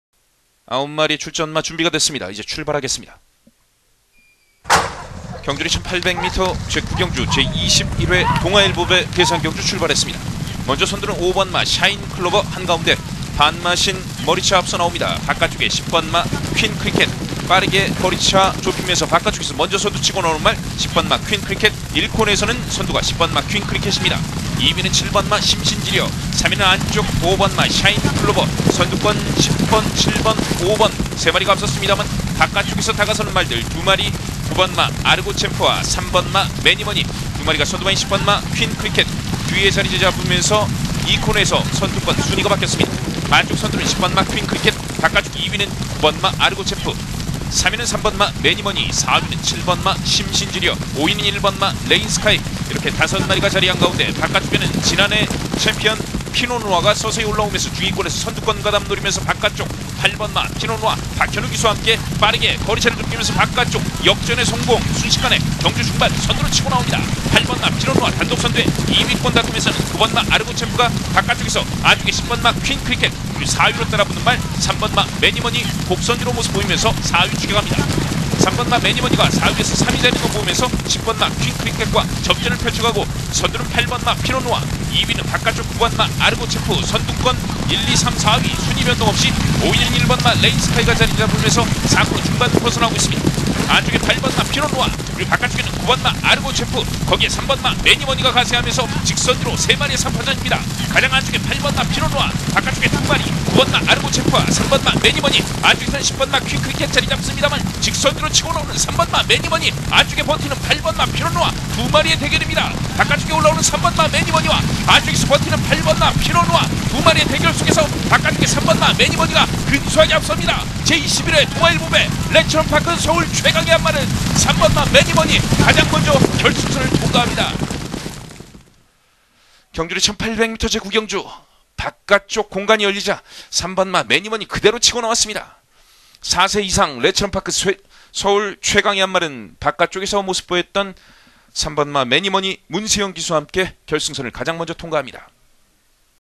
Donga Ilbo Cup 2017 - Meni Money
Meni Money (Menifee-Pocketful Of Money) wins the Listed Donga Ilbo Cup at Seoul Racecourse on Sunday February 19, 2017.

Video is from the Korea Racing Authority
More info at korearacing.live